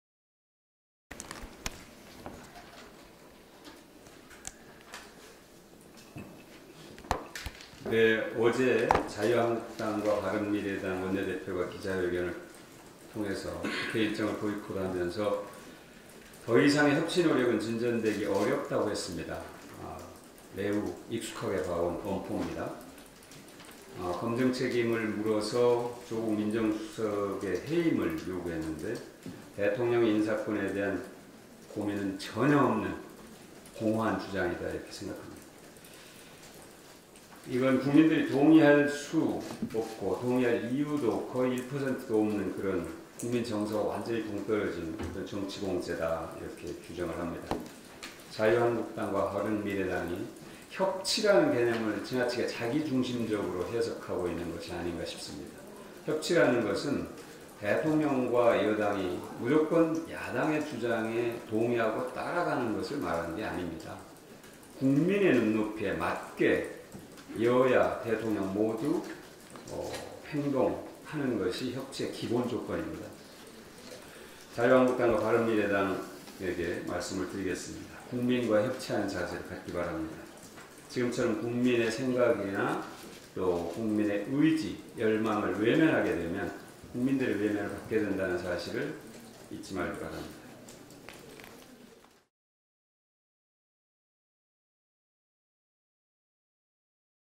박광온 "바른미래-한국당, 국민과 협치하길 바란다!"
11월 14일 국회에서 진행된 더불어민주당 최고위원회의에서는 박광온 의원이 바른미래당과 자유한국당이 협치를 자기중심적으로 해석하고 있다며, 국민과 협치하라고 말했다.